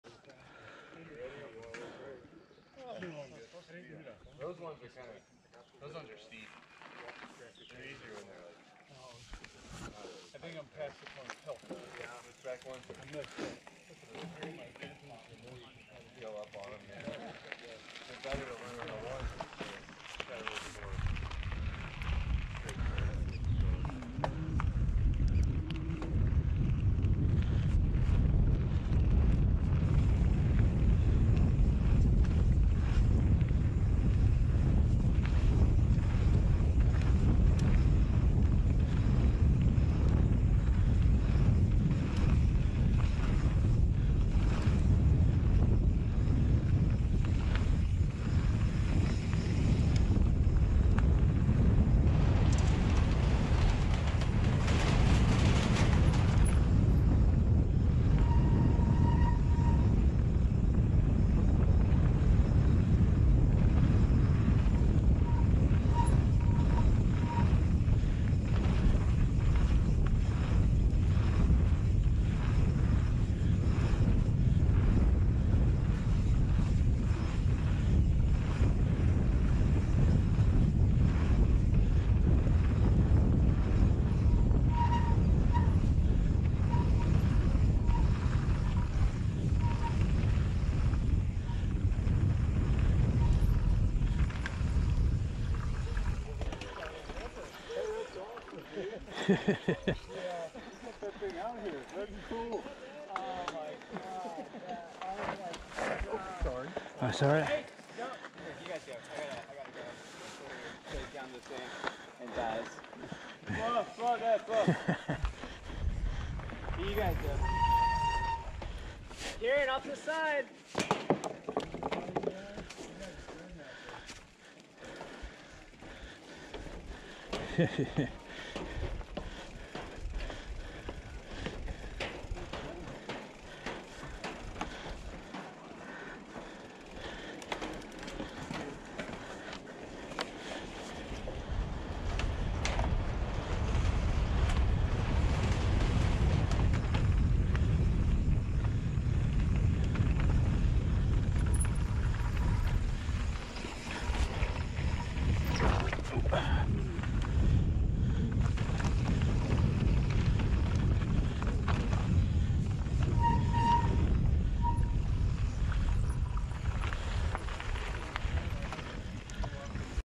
Family Flow at Coler Mountain Bike Preserve
Flowy green run in Coler. Ran into some little Rippers at the bottom of the hill.